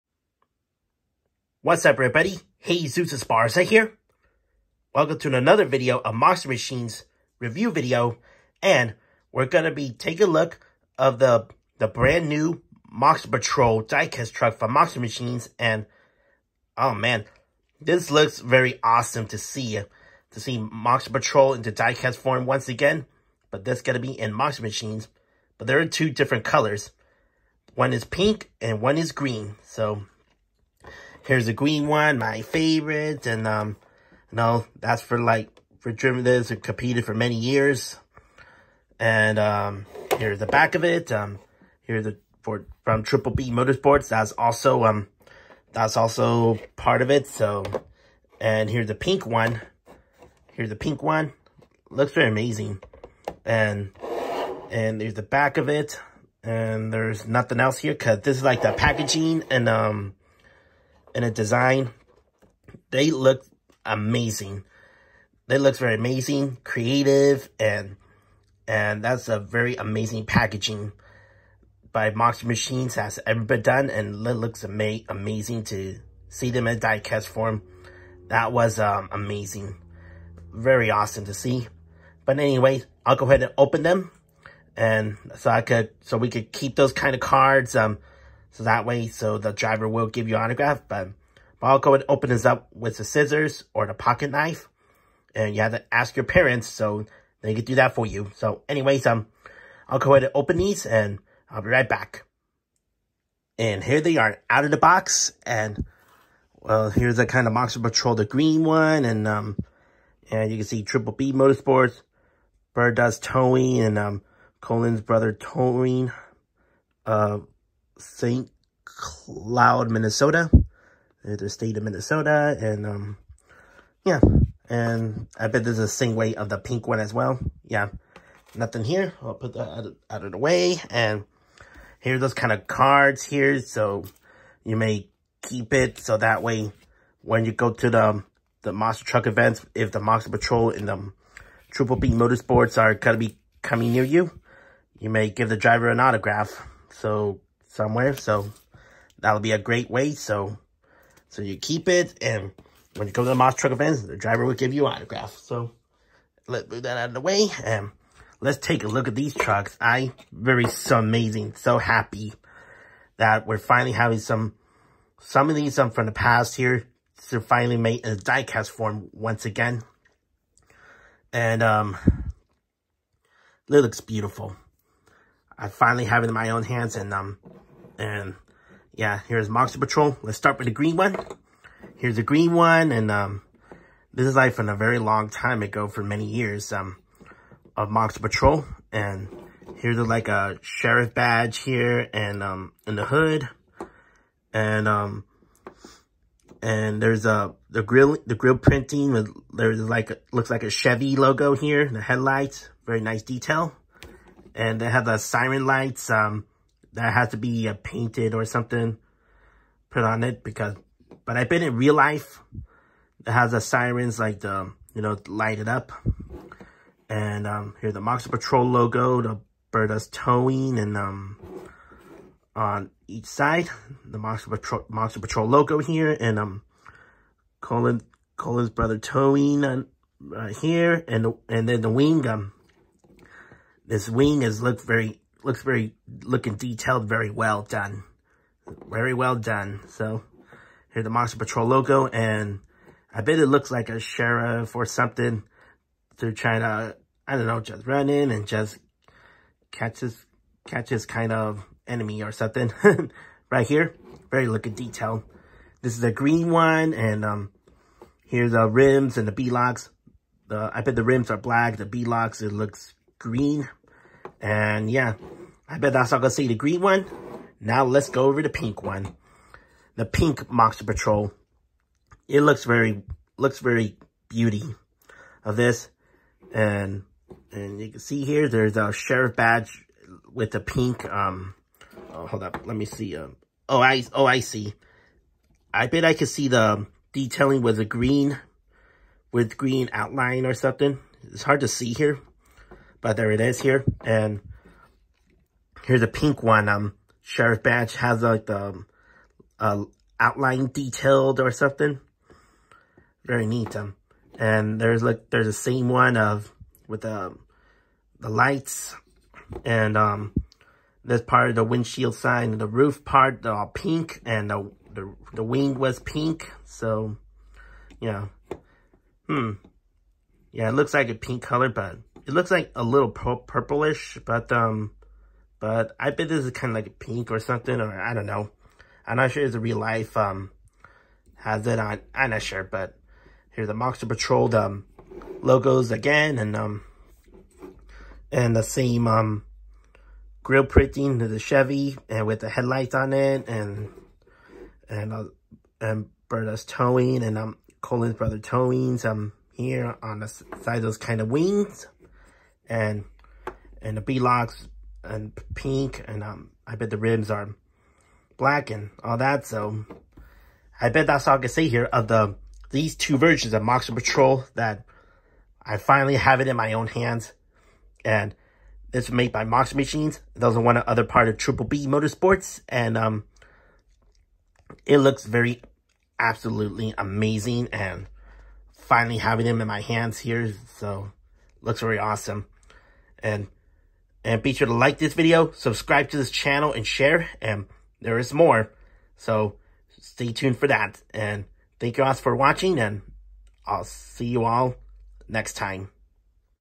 Monster Machines Monster Patrol Green and Pink Review
Another monster truck from Triple B Motorsports has one of the most classic names in the monster truck industry is Monster Patrol with 2 different color variations of Green and now Pink.